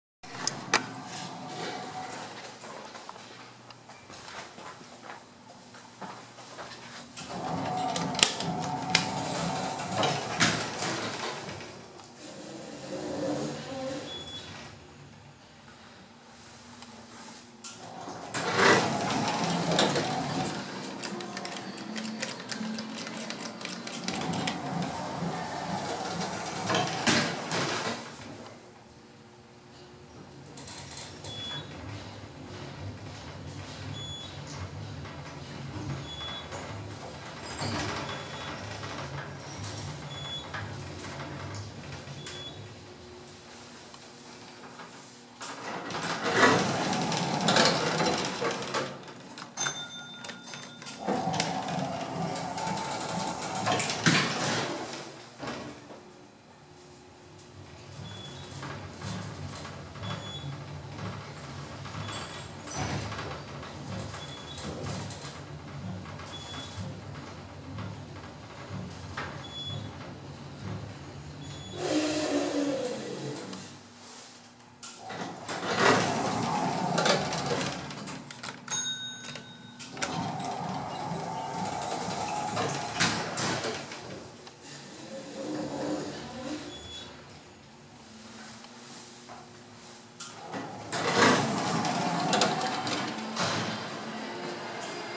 Modernized Otis Traction Elevator at 1601 3rd Ave. (Macy's) in Seattle WA
Quite similar to the elevator at Hudson's Bay, Pacific Centre in Vancouver BC. The only difference is that this elevator has EPCO fixtures instead of Dewhurst/Dupar.